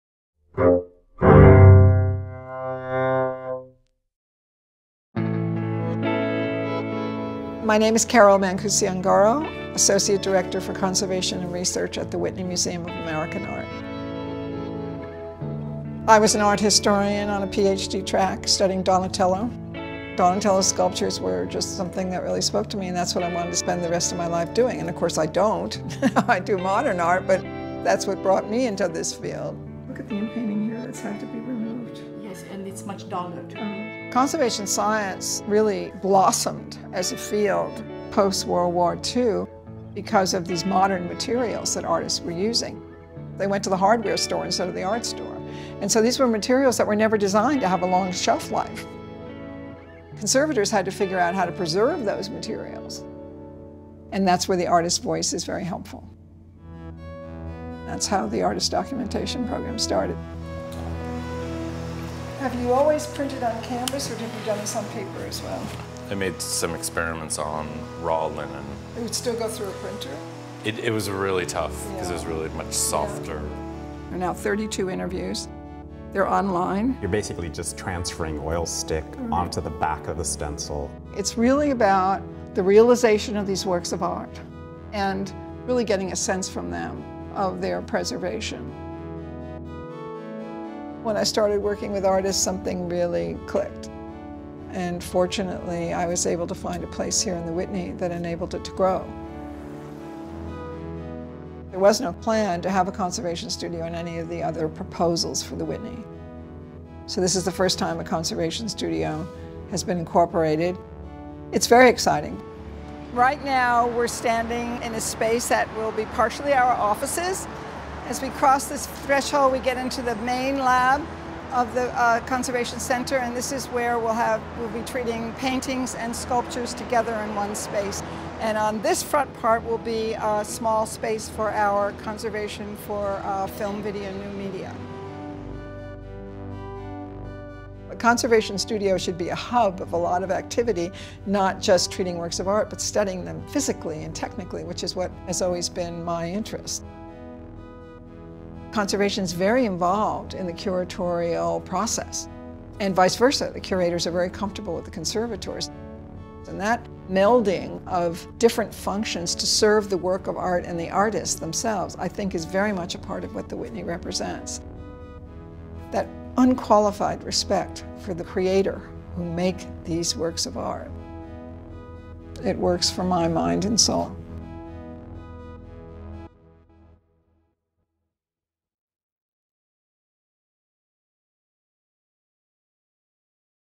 Whitney Stories: Carol Mancusi-Ungaro
In this first installment of the Whitney Stories video series, Associate Director for Conservation and Research Carol Mancusi-Ungaro discusses her professional background, the importance of the artist's voice in contemporary art conservation, and plans for the conservation studio in the new Whitney building, opening in 2015.